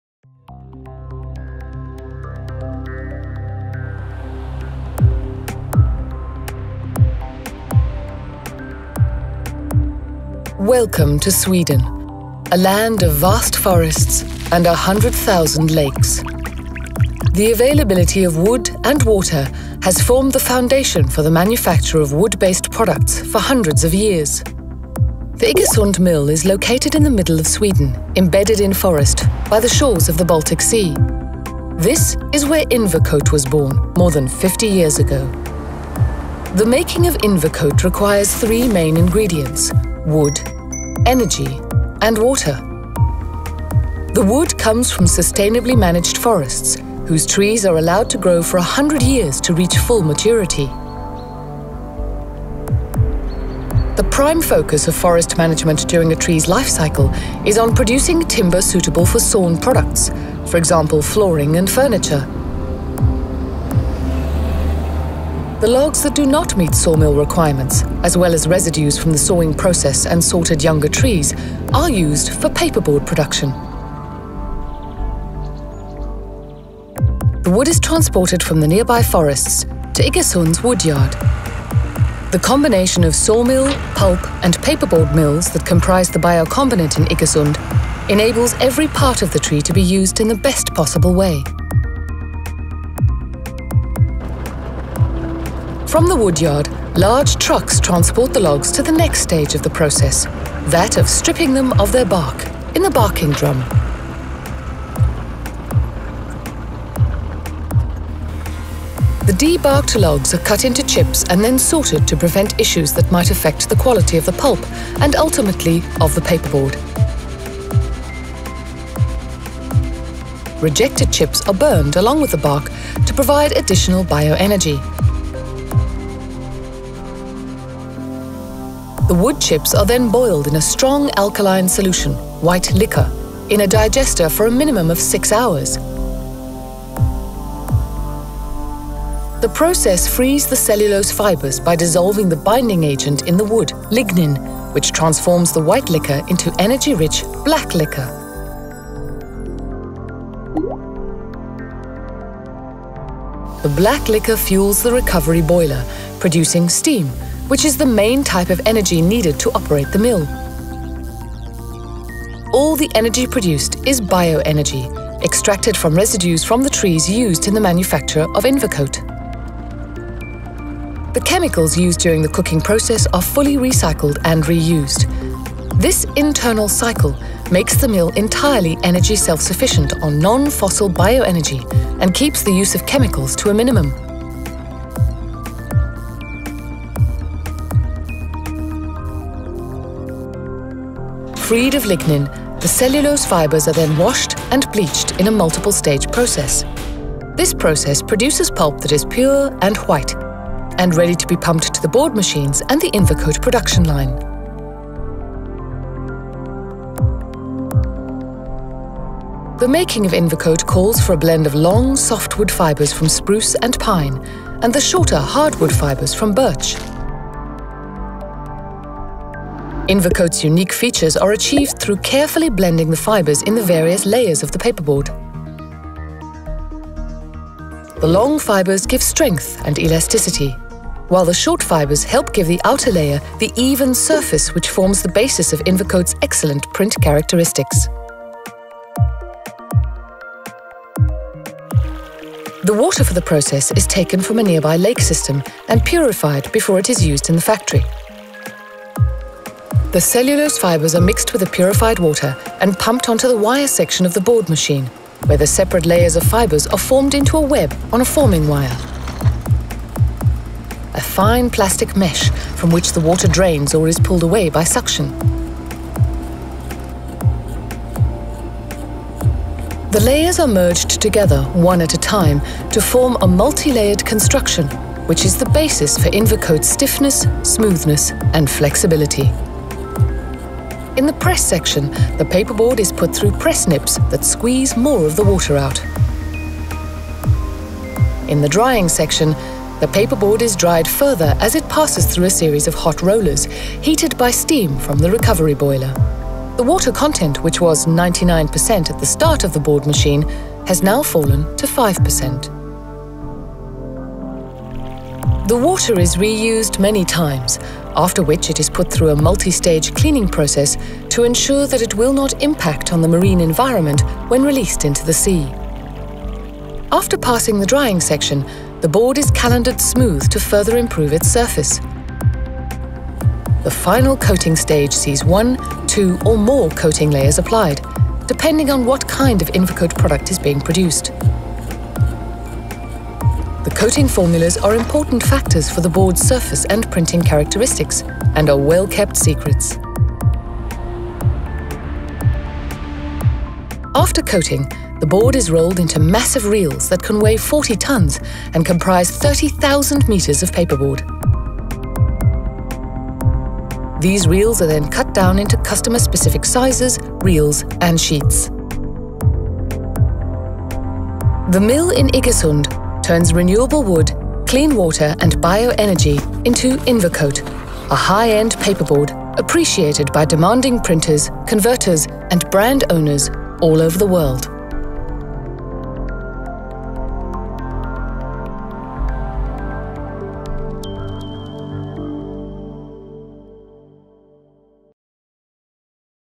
The Making of Invercote 3.0 - English
See how the leading paperboard Invercote is produced at the Iggesund Mill in Sweden. Invercote is a board extremely suited for packaging where demands on print quality and structural design are very high. High quality and consistency are key elements in the production economy for customers in both packaging and graphics.